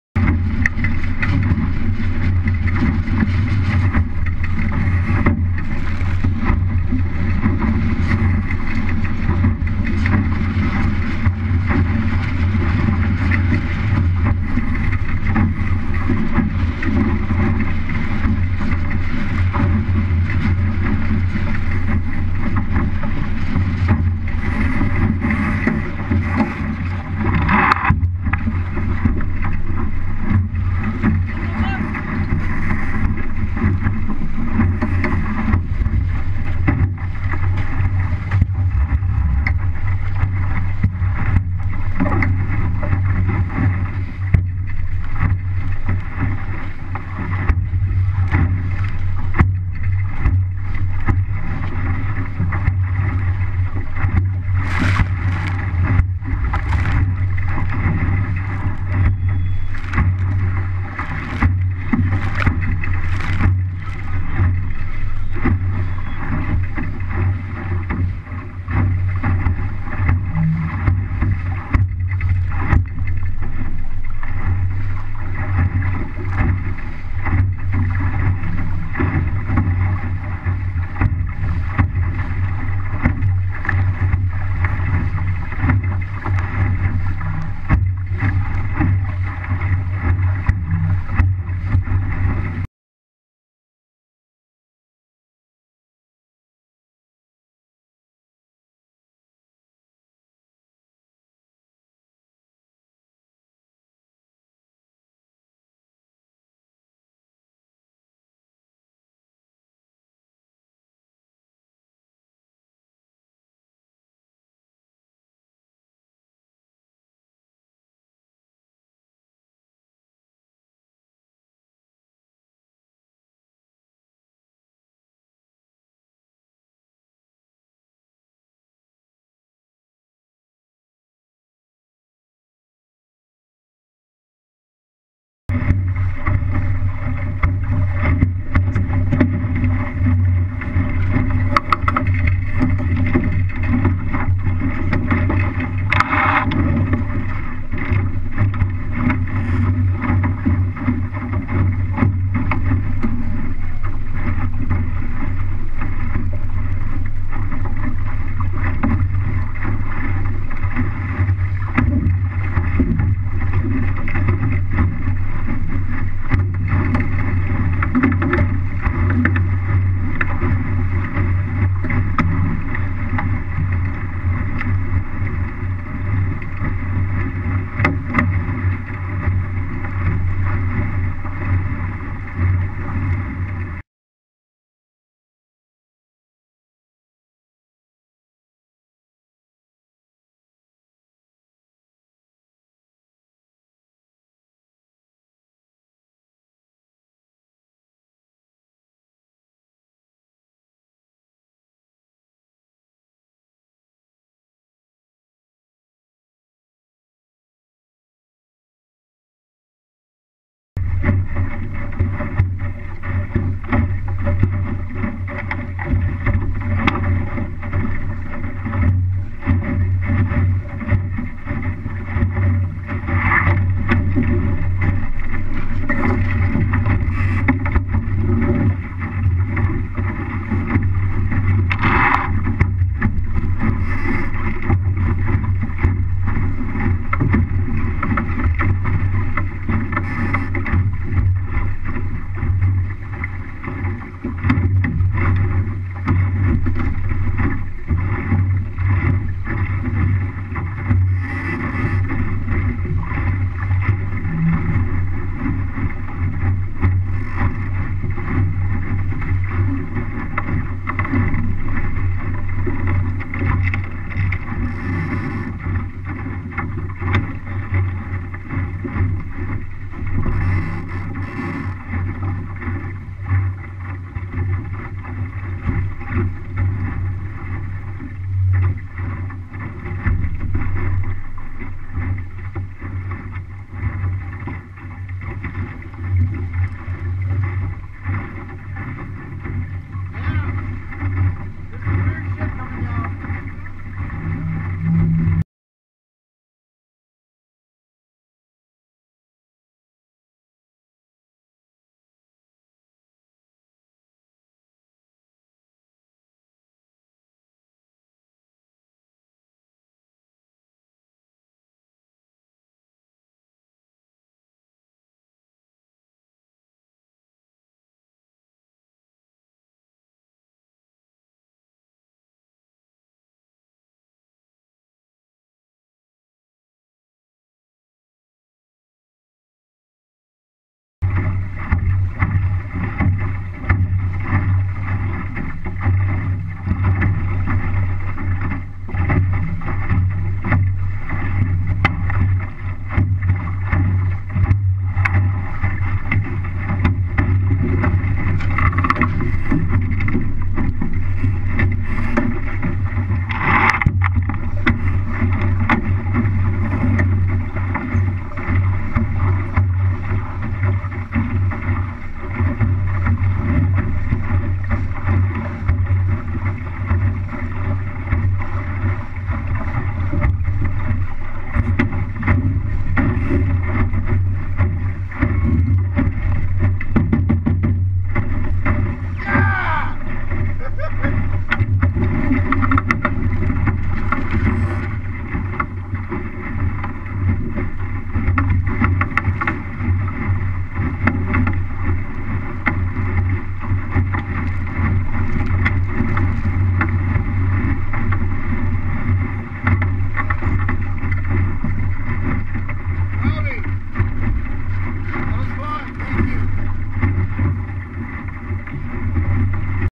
MC-Scow Midwinters Race 1
Onboard GoPro video for Chris Hawk 2470, race winner.